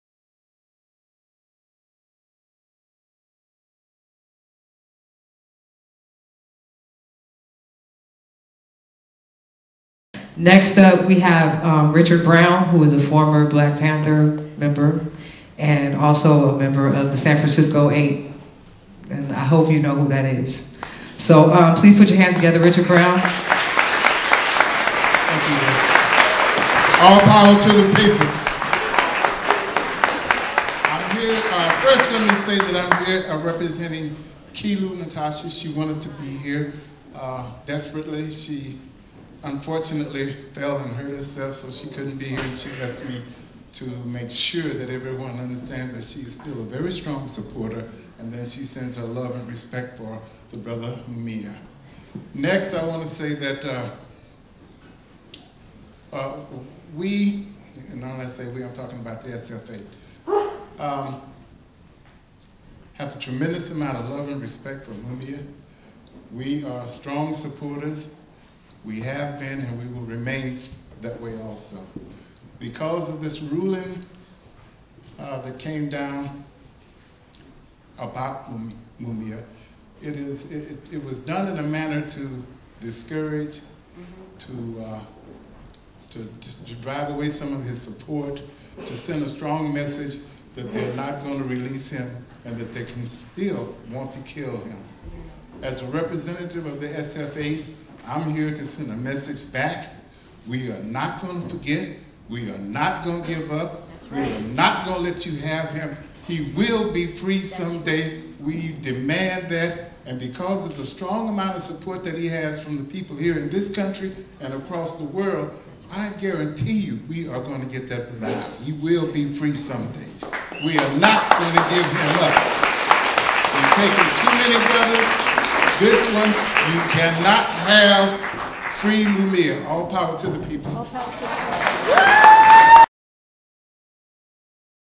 Richard Brown of the SF8 speaks for Mumia Abu-Jamal
On April 24, 2009, in Oakland, CA,  Richard Brown of the San Francisco Eight speaks at an event marking Mumia Abu-Jamal's 55th birthday and the release of his new book: 'Jailhouse Lawyers: Prisoners Defending Prisoners v. The USA'.